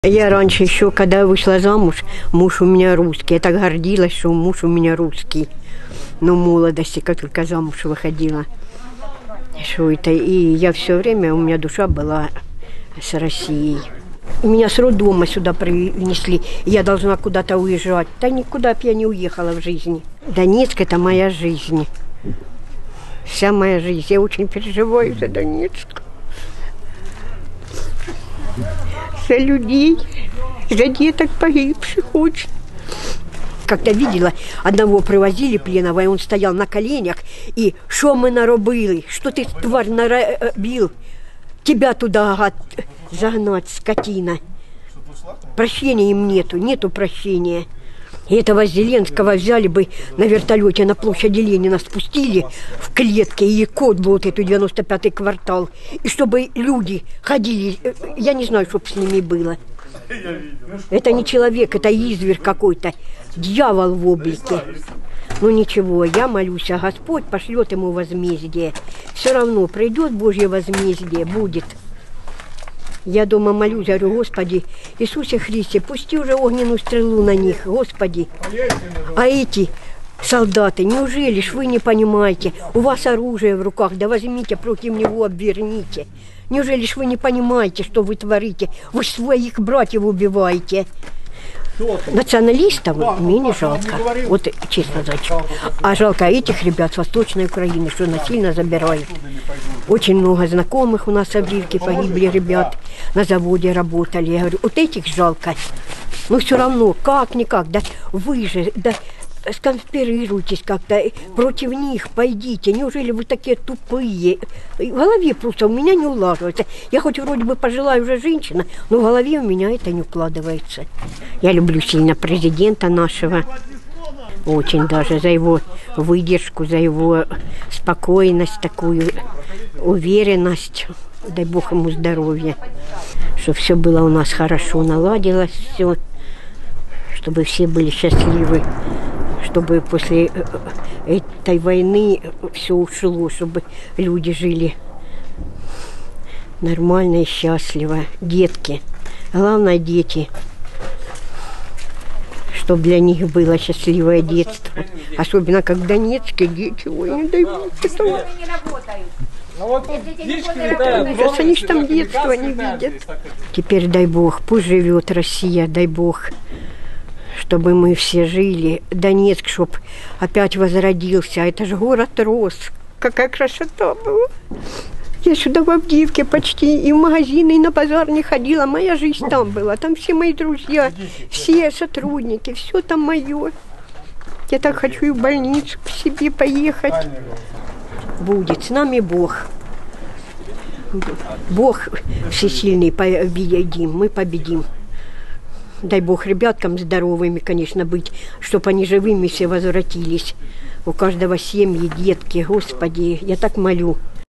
«Я так гордилась, что у меня муж русский. Почему я должна узжать?!»
Она с тяжелым сердцем вспоминает жесточайшие обстрелы со стороны ВСУ. «Нет вам прощения. Вы же своих братьев убиваете», — в слезах говорит пожилая женщина.

Колоссальные разрушения и постоянный страх — вот, что оставляют после себя украинские боевики.

«Я люблю нашего президента, дай Бог ему здоровья! Я верю, что у нас все наладится. С нами Бог, мы победим!».